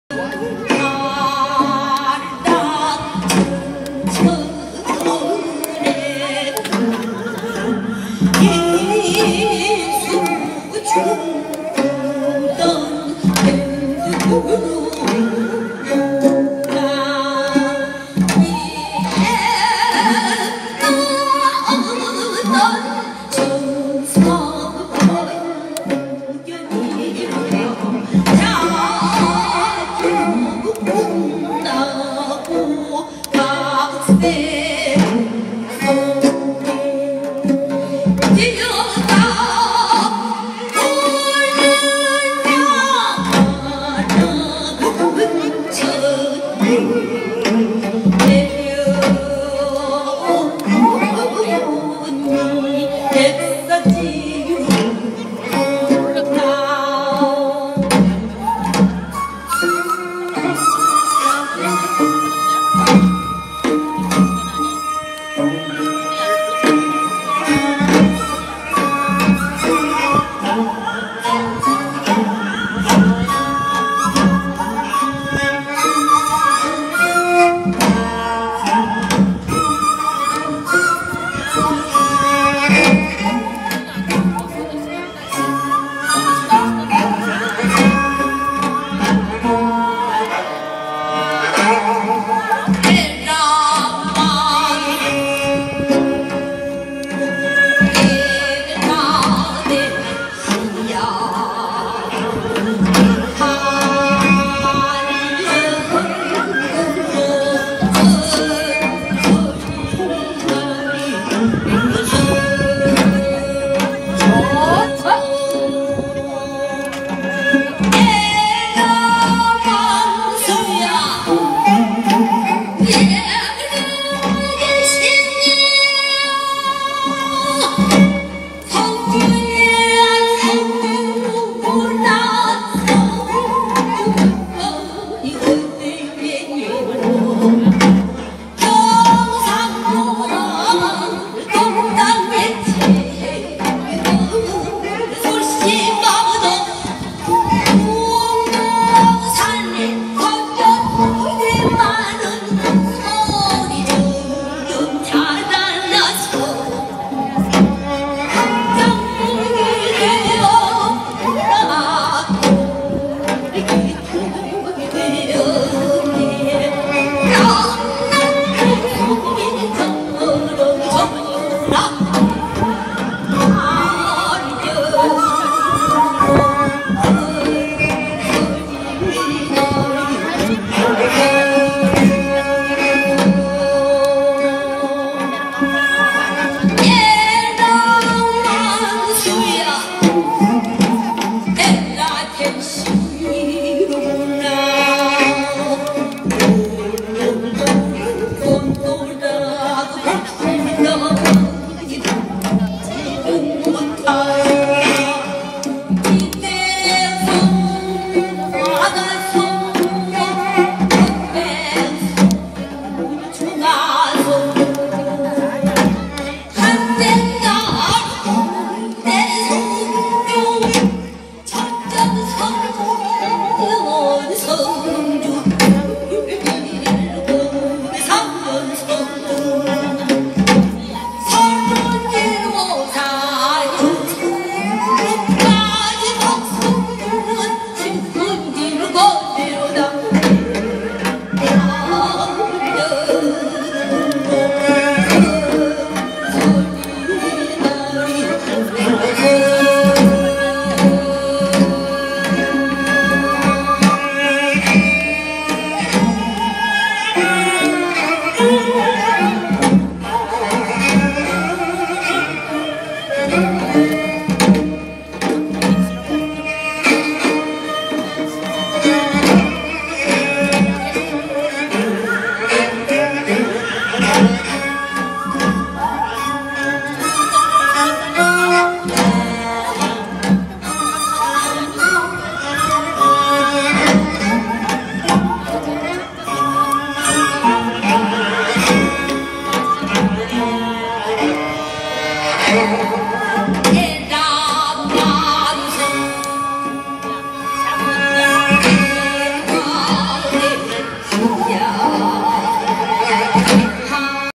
💕🎤🎶2019.6.15(토)오후7시 국동 다기능항 특설무대에서~♡토요상설공연 ''힐링뮤직단''《입 춤》 ☞혜화무용단-한국무용!!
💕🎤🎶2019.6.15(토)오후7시
국동 다기능항 특설무대에서~♡토요상설공연
''힐링뮤직단''《입 춤》

☞혜화무용단-한국무용!!